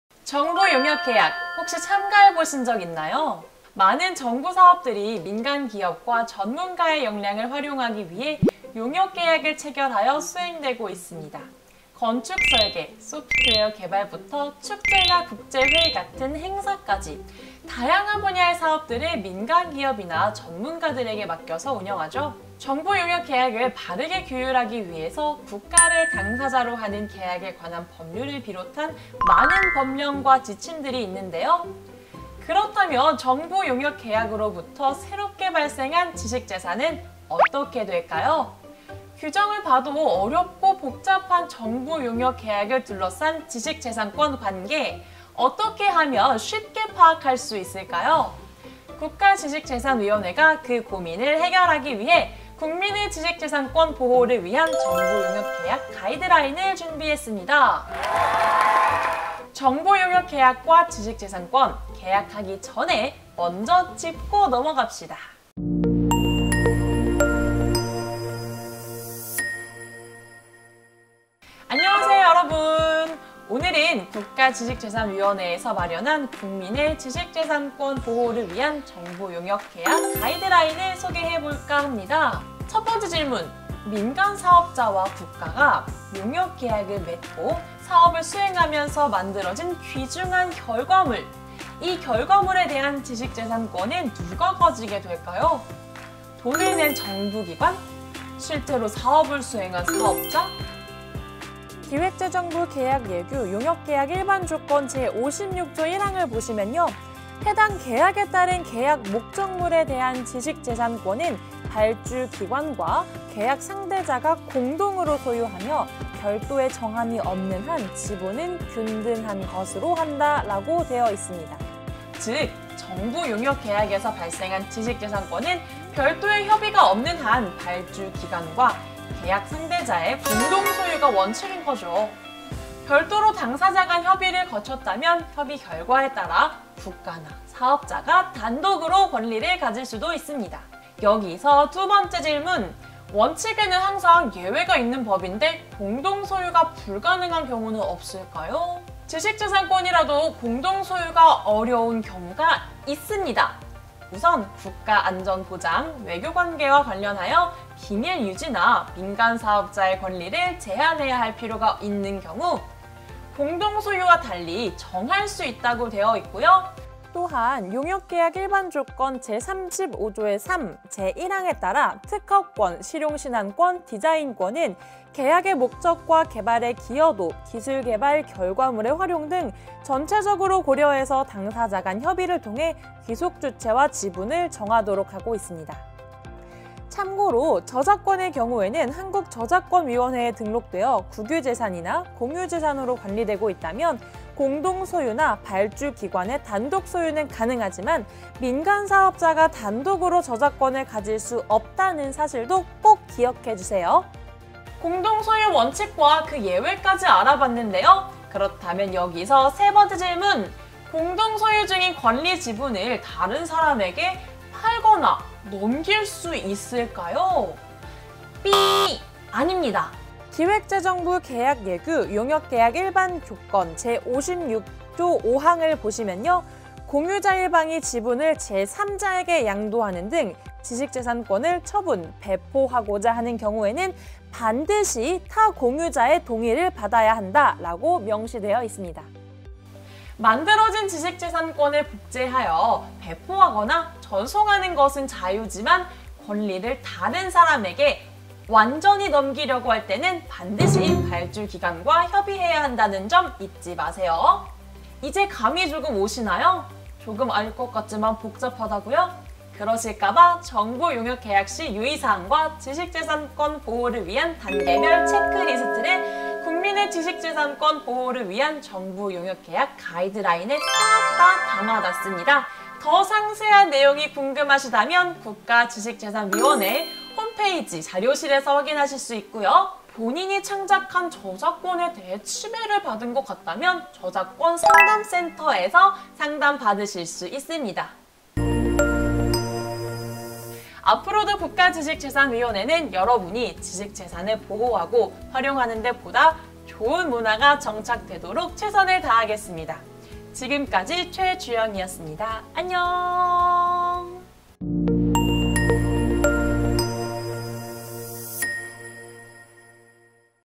정부용역계약 가이드라인 홍보영상(feat. 국가지식재산위원회)
정부용역계약을 통해 새롭게 발생한 지식재산은 어떻게 해야할까요?
국가지식재산위원회에서 이러한 궁금증을 해소하기위해
정부용역계약 가이드라인을 준비했습니다. 함께 보시죠!